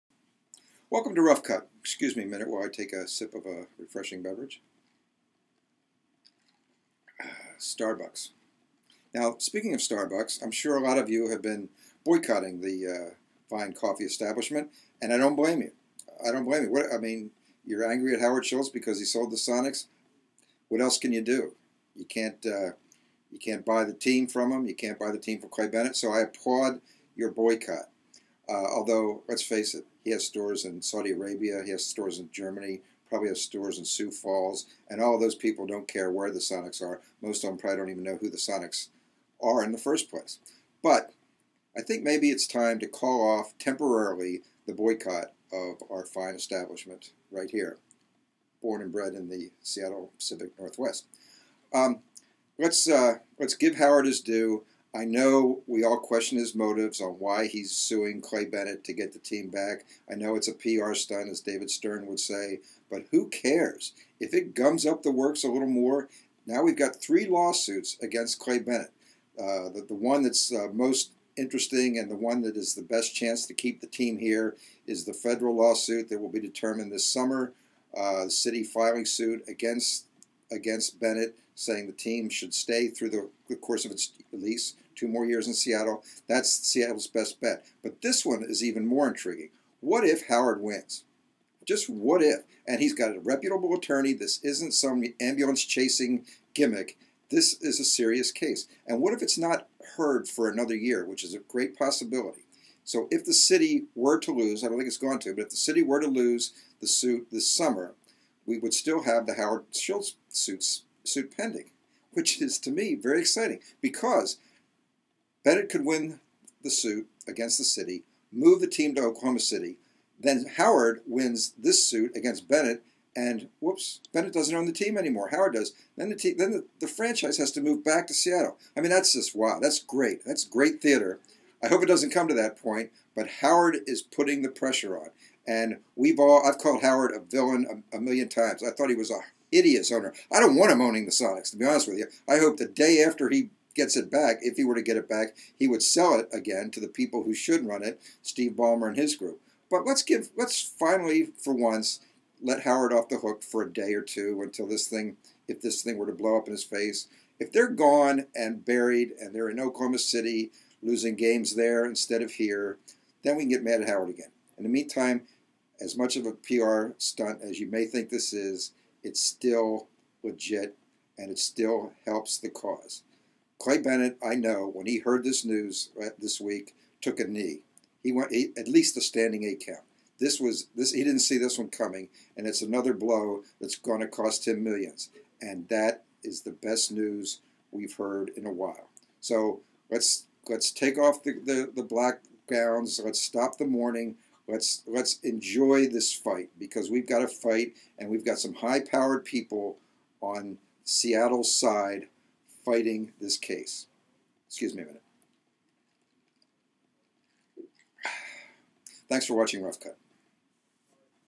Steve Kelley's Rough Cut
Seattle Times columnist Steve Kelley talks about the Sonics owners -- both old and new -- as Seattle's NBA franchise faces a potential move to Oklahoma City.